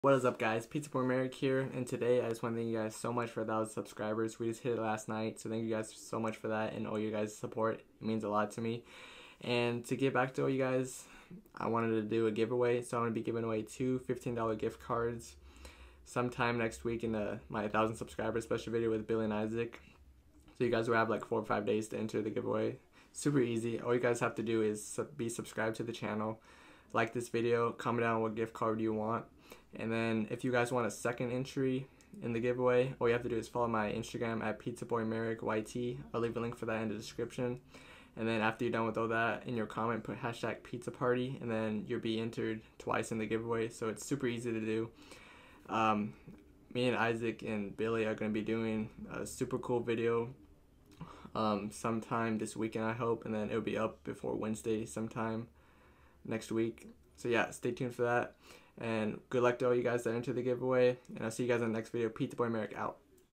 1,000 SUBSCRIBER GIVEAWAY💯🥳
Hey guys I wanted to give back to you guys with a Giveaway!! Just like the video said you have to follow these steps to be entered!
2. Like this video💯
3. Comment what gift card you want!
Also put PizzaParty🥳🍕 in your comment when your all done good luck everyone🤩💯